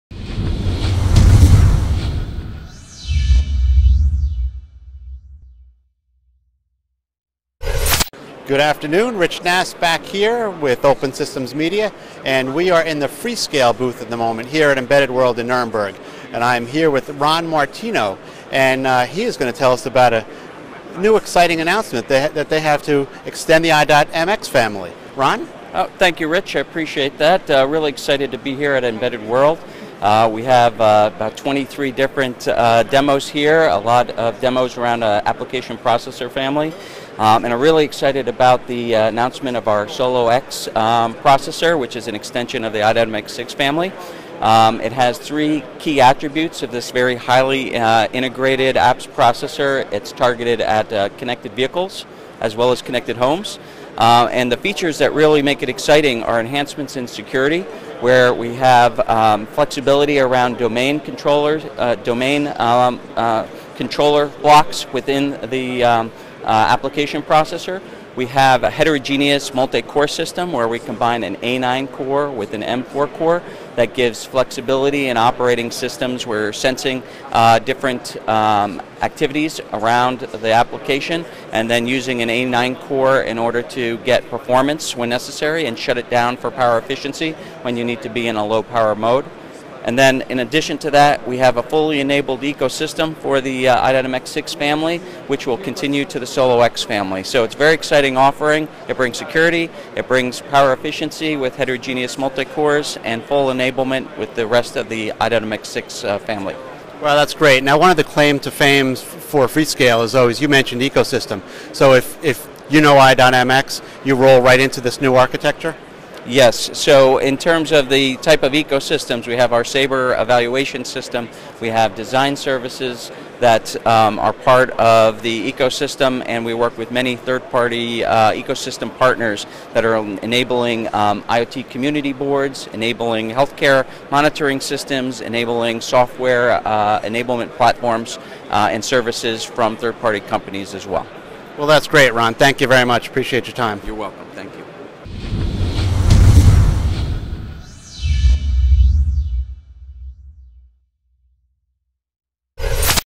Embedded World 2015 – Ronald Martino, Freescale Semiconductor
Rich Nass interviews Ronald Martino VP of Application Processors and Advanced Technology at Freescale @embedded_world